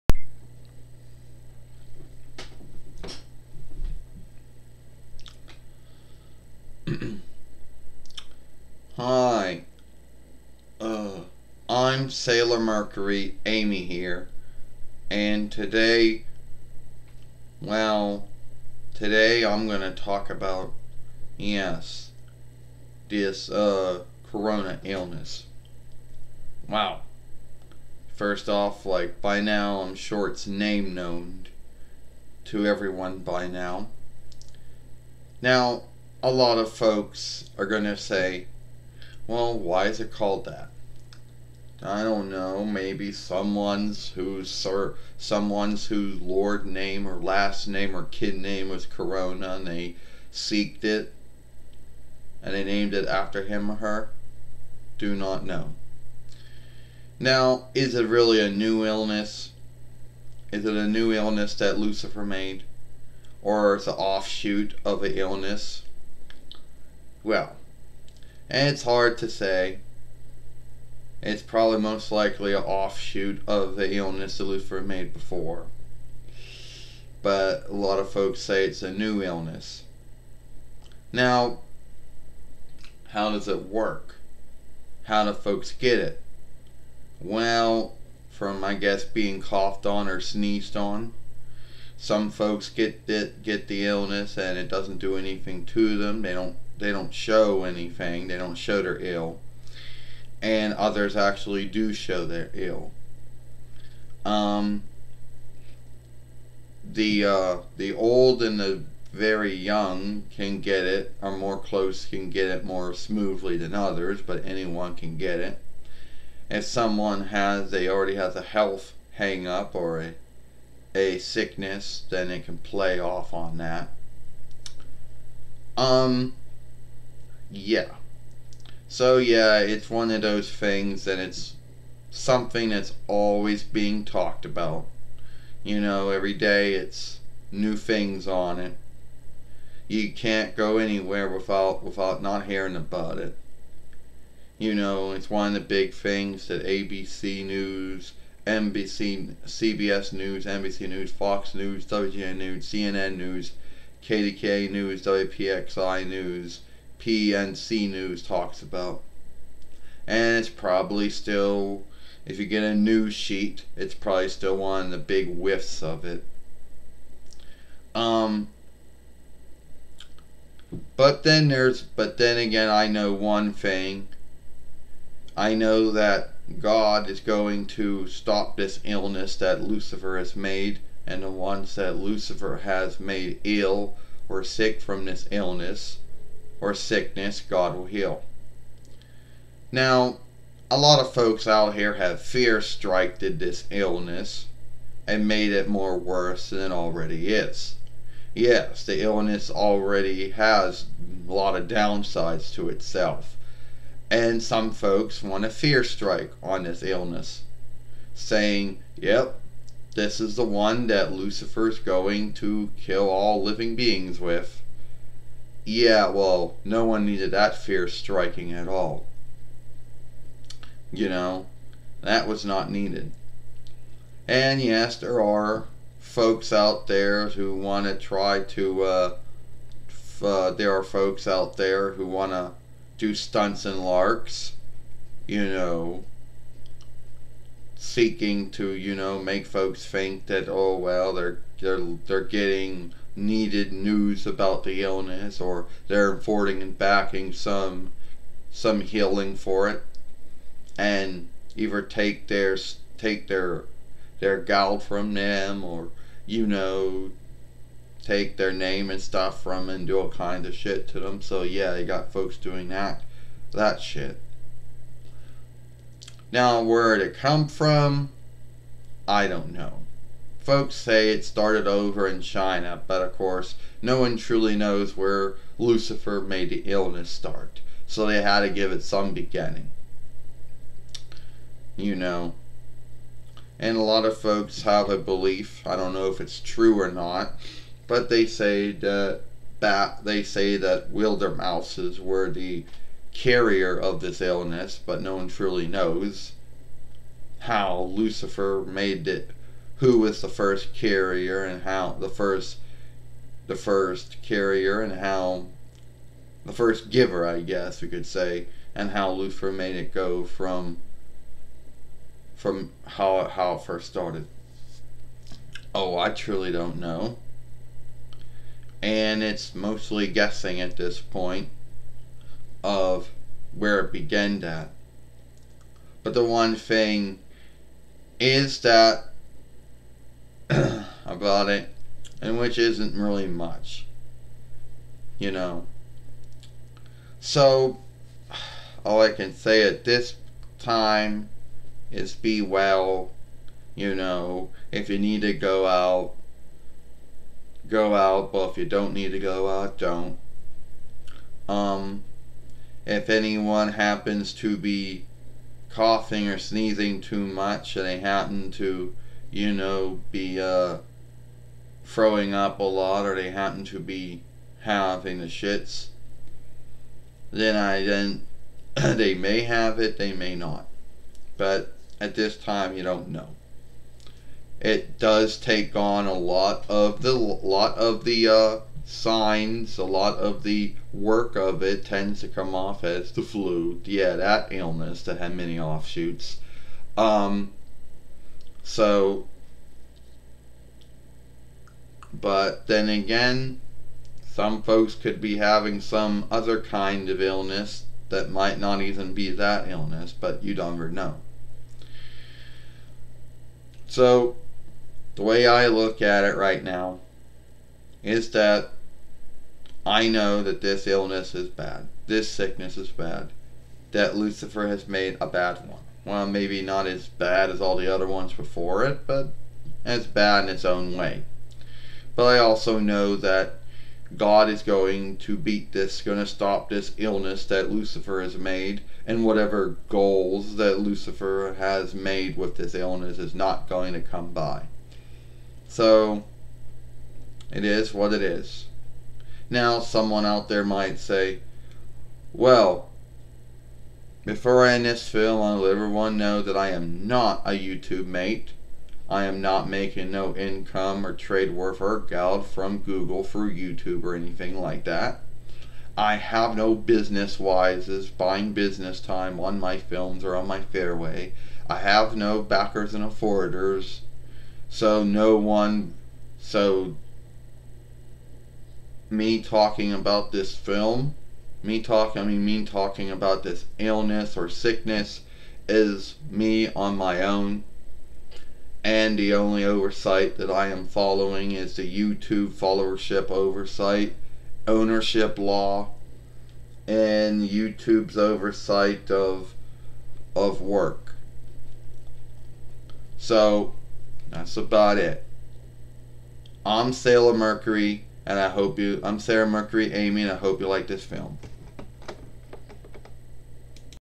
SAILOR MERCURY TALK ABOUT CORONA ILLNESS
SAILOR MERCURY TALK ABOUT CORONA ILLNESS THAT LUCIFER HAD MADE AND GOD IS GOING TO BEAT CORONA ILLNESS  AND TALKING ABOUT THIS ILLNESS